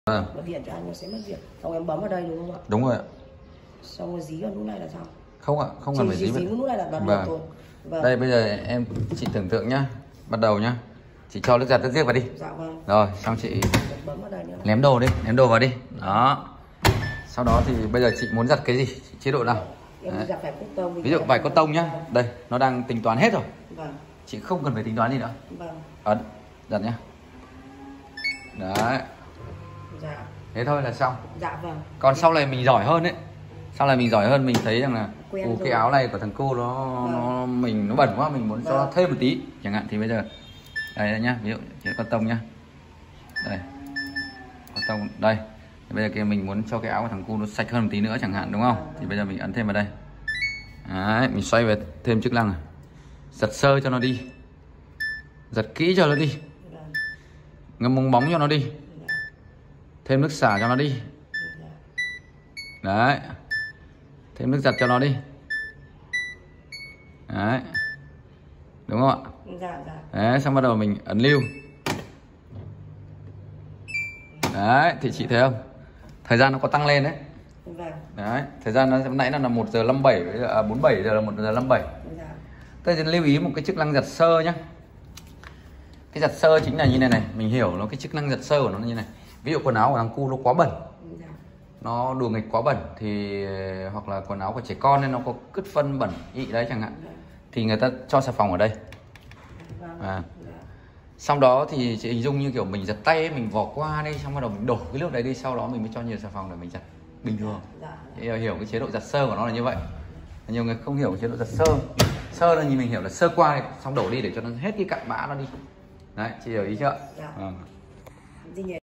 Hướng dẫn giặt chế độ cottong nhanh và dễ hiểu nhất|samsung dientuluudong Samsung AI
Hướng dẫn giặt chế độ của máy giặt thông minh AI đơn giản dễ hiểu nhất#dientuluudong samsung setup maygiat samsung AI